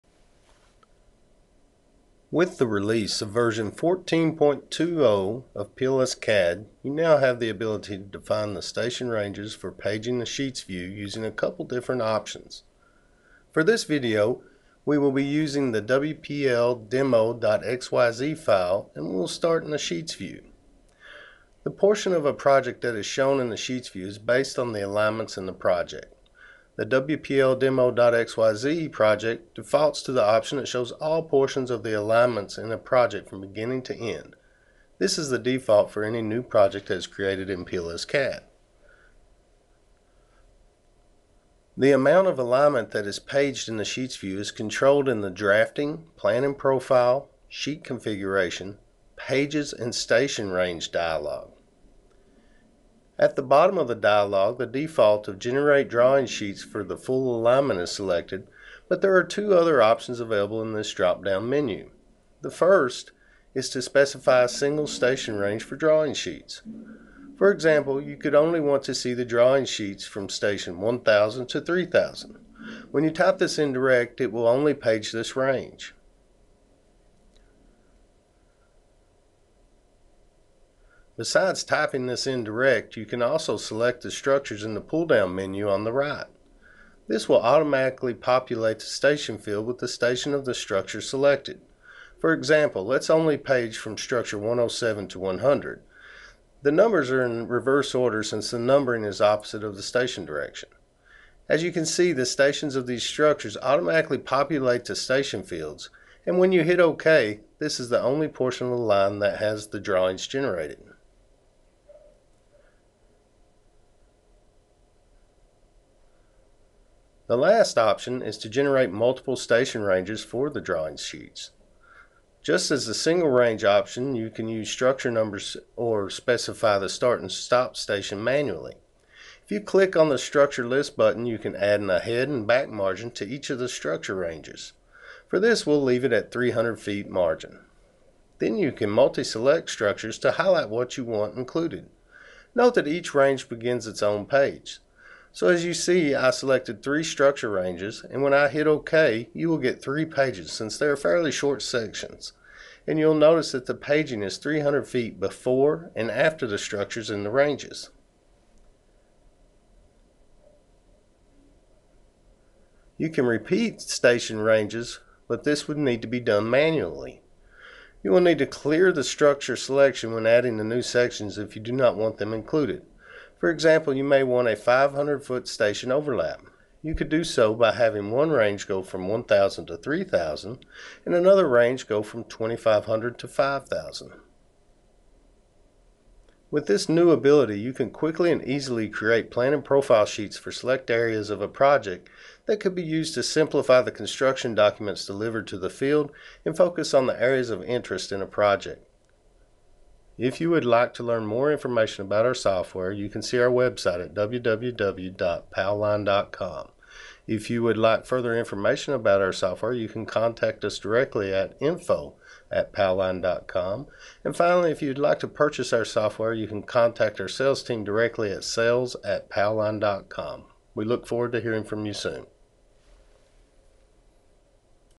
PLS-CADD Sheets Station Range Selection
Demonstrates the new options for single and multiple paging station ranges when creating Plan & Profile sheets in PLS-CADD.